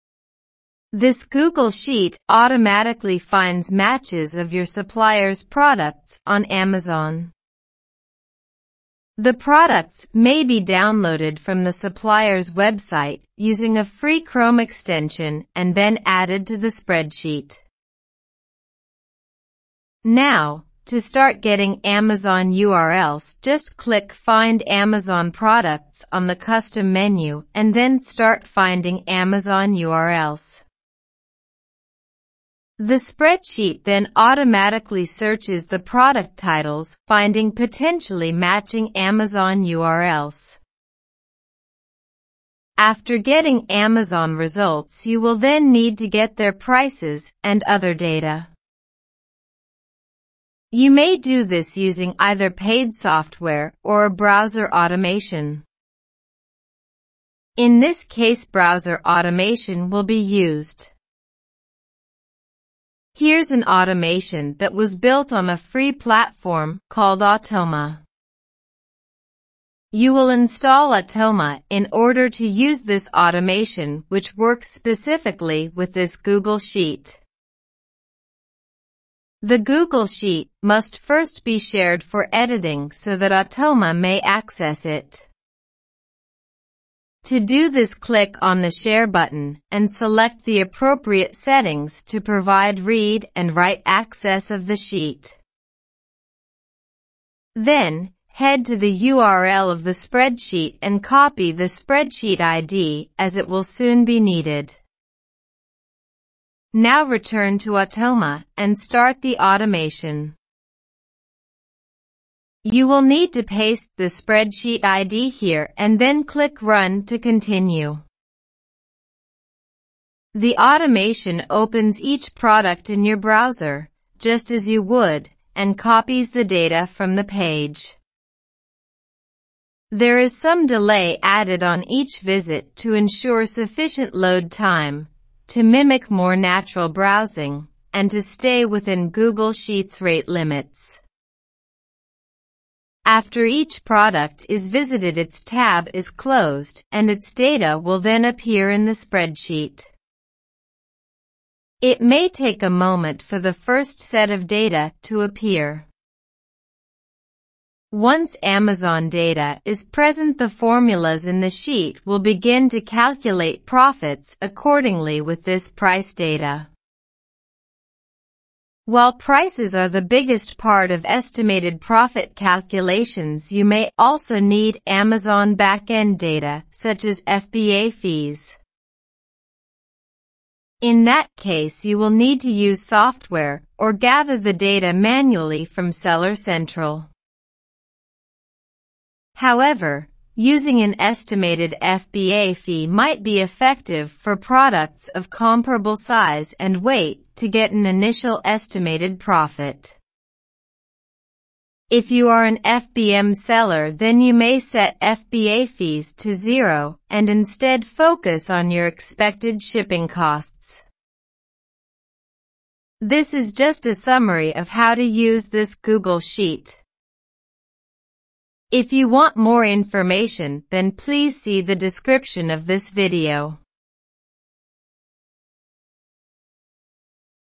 Match Suppliers Products to Amazon Automatically in Google Sheets
As the title suggests you can automate your product sourcing for Amazon with this Google sheet. This one works with a supplier products list and finds matches on Amazon. Then using browser automation we can actually get the Amazon data without paid software.

Optionally you can also use paid software to get the back-end data such as FBA Fees. Keep in mind that there are some products in the Amazon catalog that do not have a preexisting FBA fee. I have seen this only on products where no FBA sellers are present. My guess is that they have never been sold FBA before. So in these cases you may still need to use the FBA calculator on seller central. Another option is to use an estimated FBA fee if the products are all basically the same size and weight. But this of course is not exact. For FBM, set FBA fee to 0 and just add your own expected shipping.

Go here to see a more complete demo and get a copy of this Google sheet: